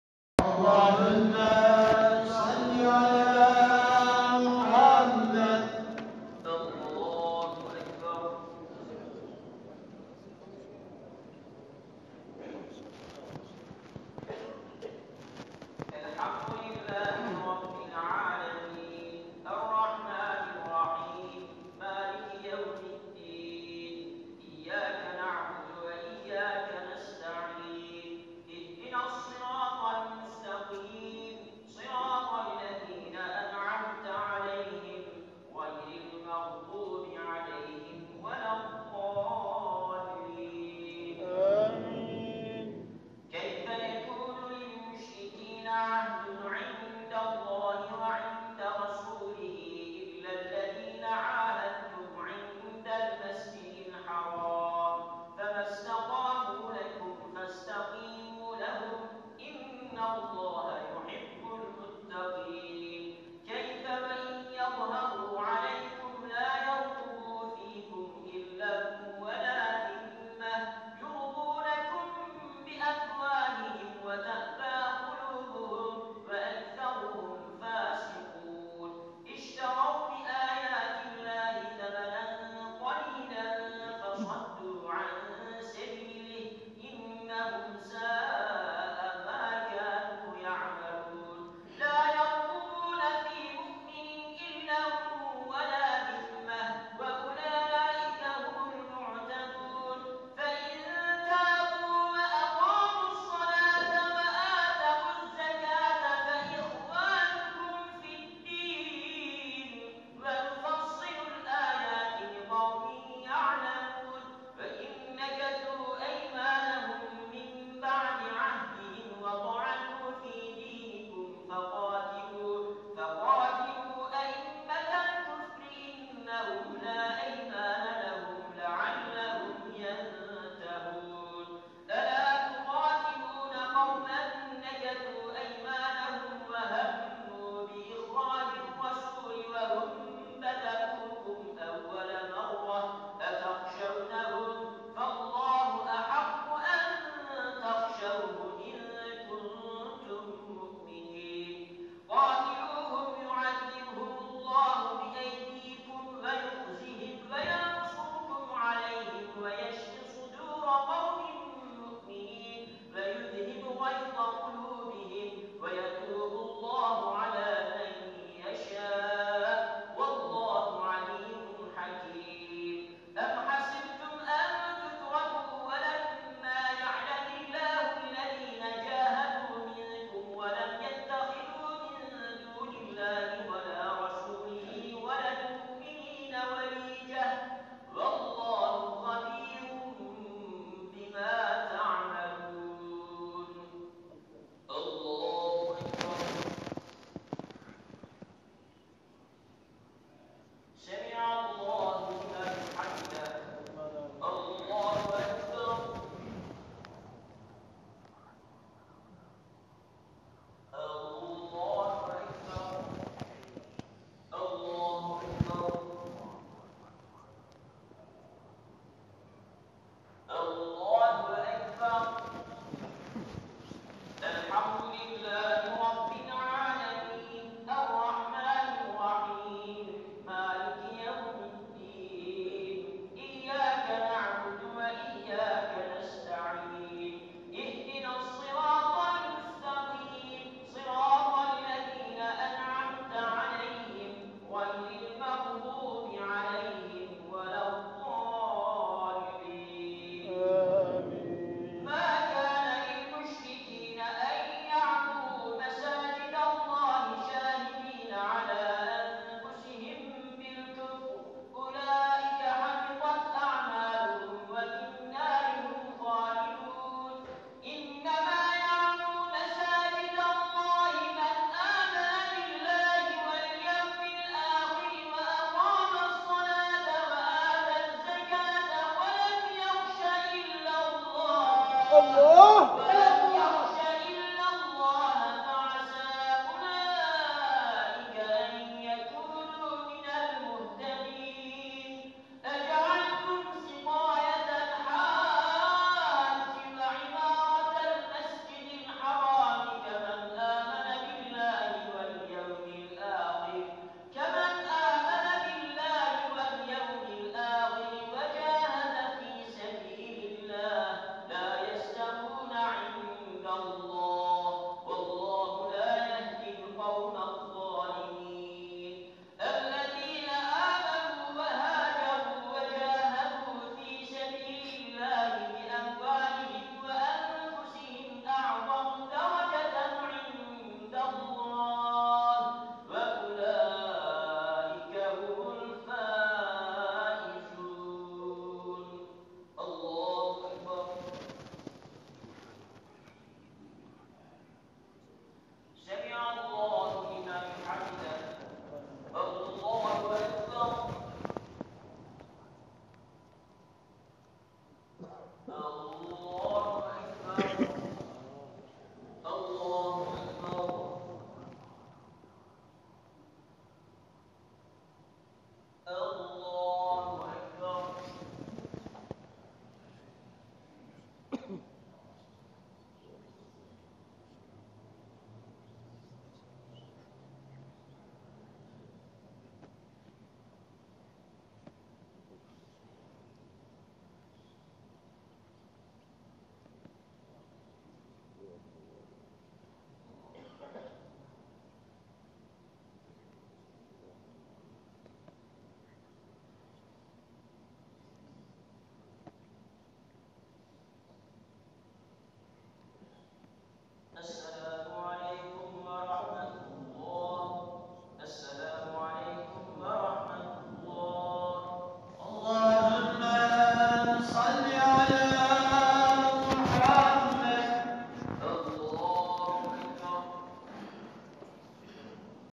Pendik Sahil Camii Teravih Namazı (04:41)E DİKKAT
Mehmet Hocamızın muhteşem sesiyle teravih kılmayan hâlâ varsa; bu Ramazan ve her Ramazan Pendik Sahil Camii'ne bekliyoruz. (4:41) e dikkat ! Sadhezar Maşaallah ..!